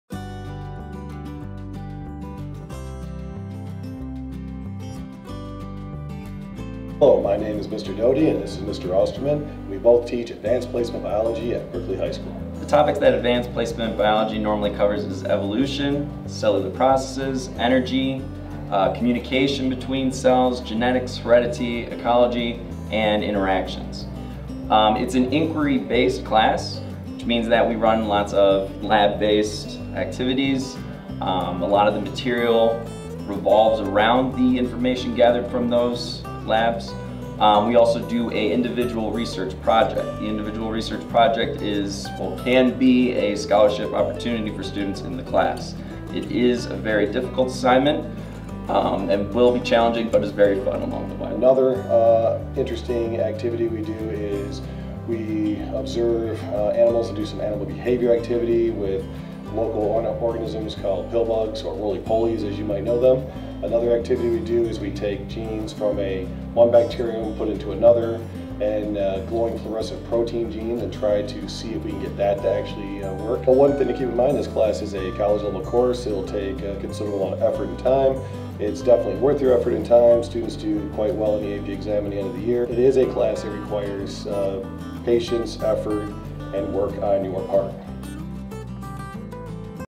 BHS AP Biology
Learn about AP Biology at Berkley High School from Mr. Doty & Mr. Austermann.